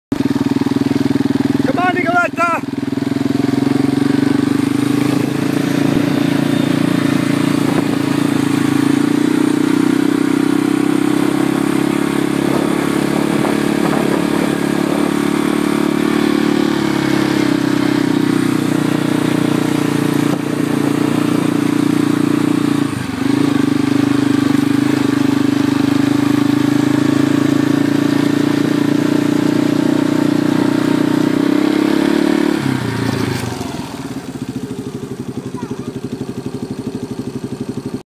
Nicoletta Quad Riding
Nicoletta learning how to ride her quad in fields.